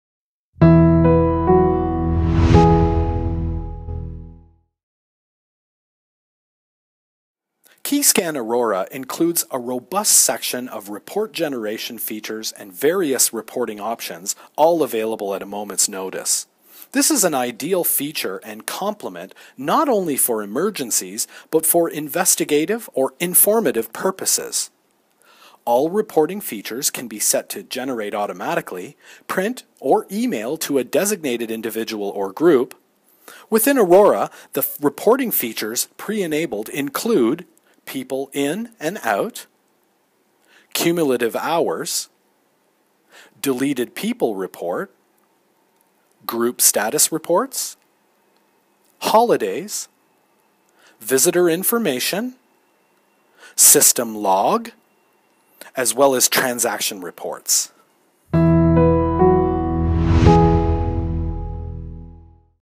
Keyscan Aurora - Reporting
Keyscan Aurora is built with multiple and robust reporting options for site or system-wide transactions, reader access, people and group status, scheduling, report generation options and many more. Sharing reports is made easier with a number of export file formats to choose from.